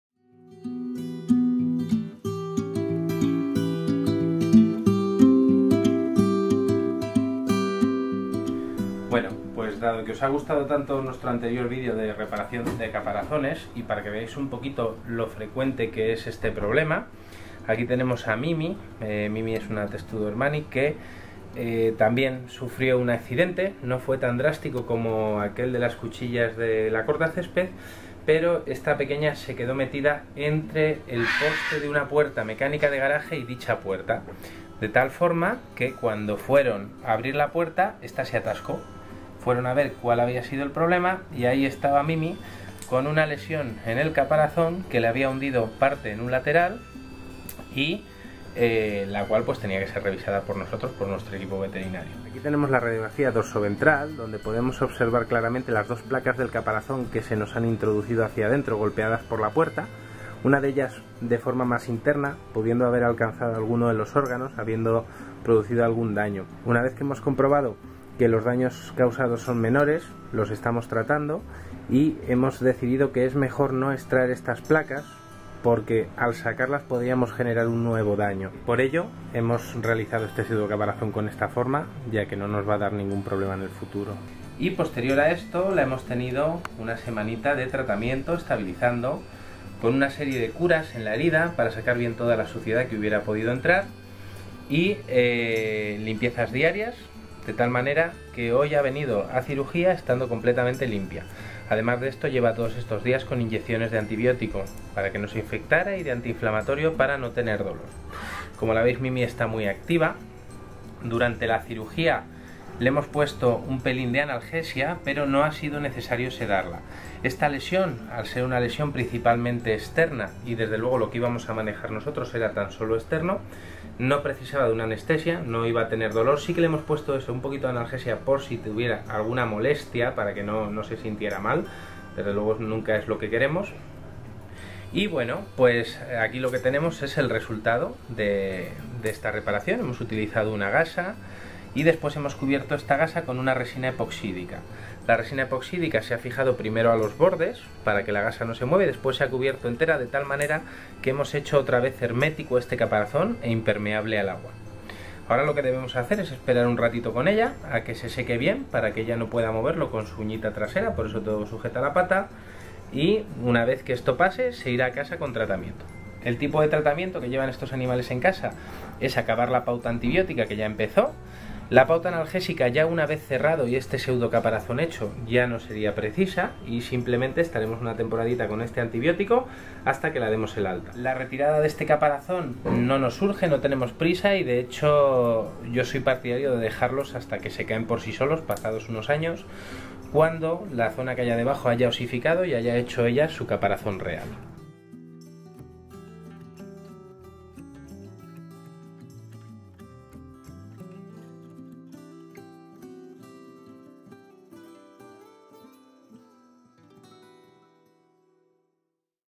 CAP 175. Medicina de reptiles. FRACTURA del caparazón de una tortuga de tierra.
De nuevo otra TORTUGA CON FRACTURA DE CAPARAZÓN
En el vídeo de hoy Javier Garcia Javier García Hernando, uno de nuestros veterinarios especialistas en animales exóticos, nos cuenta el caso de Mimi, una tortuga que acude a nuestro Hospital tras sufrir una fractura de caparazón por un accidente con la puerta de garaje en la que quedó atascada. Son muchas las tortugas que acuden a nuestros hospitales por este problema, ya sea por mordeduras de perros, accidentes domésticos (cortacésped), aplastamiento…
El caparazón de las tortugas puede representar hasta 2/3 del peso total del animal. Está compuesto por dos partes: Dorsal (o caparazón) y ventral (o plastrón), estas dos partes están unidas por puentes óseos.
Cuando se presenta un animal con ese tipo de traumatismo, lo primero que tenemos que hacer es estabilizarle. Después, realizar un radiografía es fundamental para conocer el estado de los pulmones y el contenido abdominal. En el caso de Mimi hemos comprobado que los daños internos son menores, por lo que no será necesario retirar las placas que han sido desplazadas.
Volveremos a ver a Mimi en revisiones periódicas, mientras en casa seguirá con tratamiento, pero podemos ver que tras la cirugía se encuentra bastante activa.